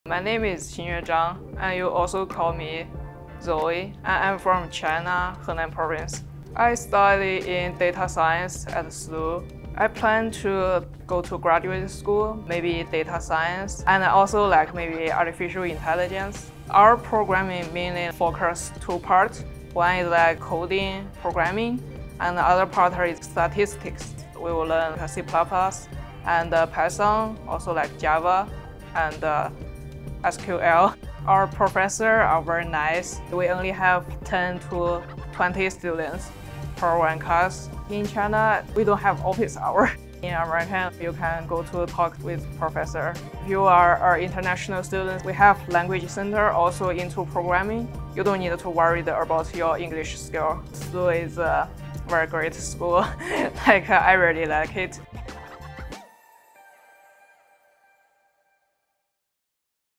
Zoe from China | Studying Data Science at SLU
As part of her graduate school plans, Zoe traveled to the US to study at Saint Louis University, with the goal of continuing her studies in data science or AI. Zoe talks about the benefits of studying abroad and how students can get language and educational support.

Learn more about studying at INTO Saint Louis University on our